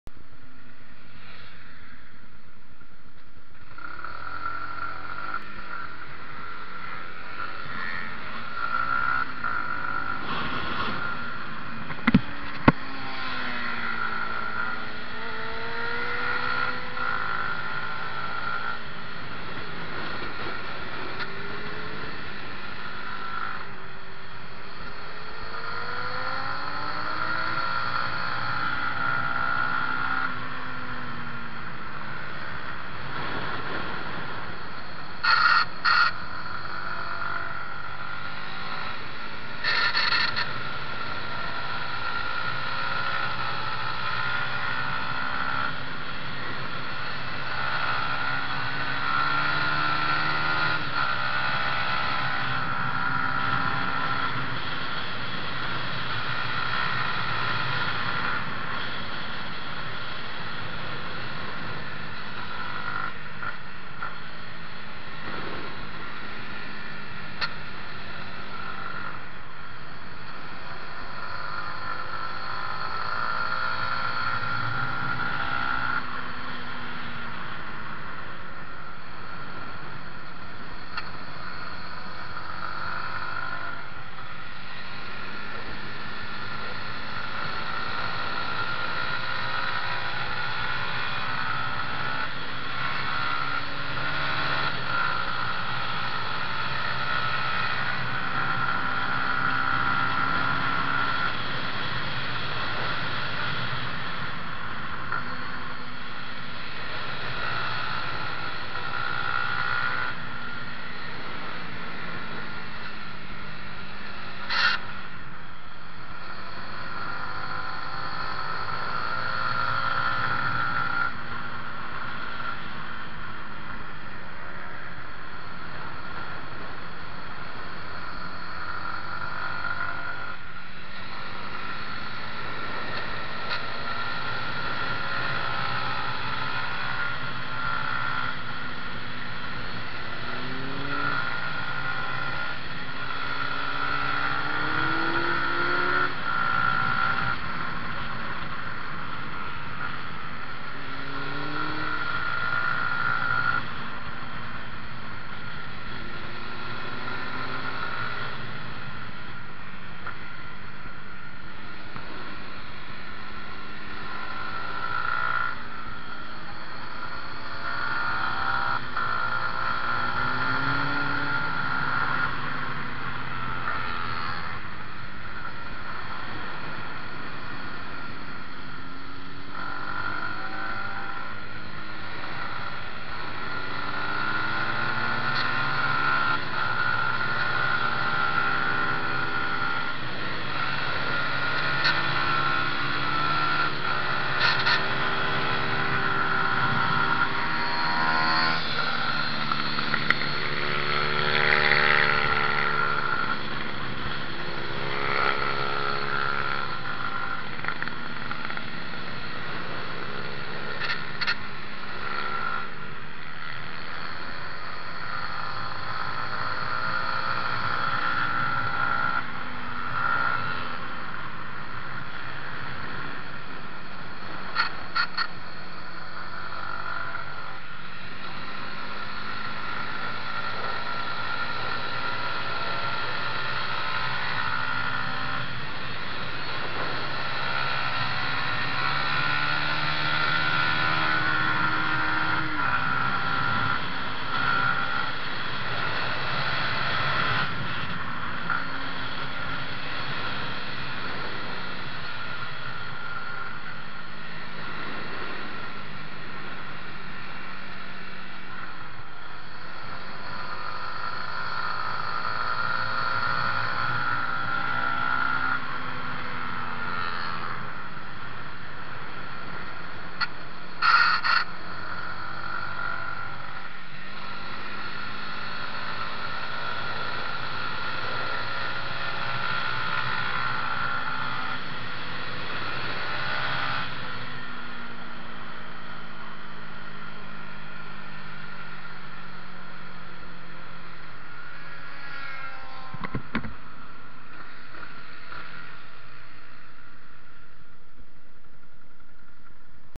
Torowanko na Radomringu cz. 2 MT-125 2020
Torowanko na Radomringu, czyli Autodrom Radom. Pogoda dopisała, oponki w Czarnym Mietku robią robotę.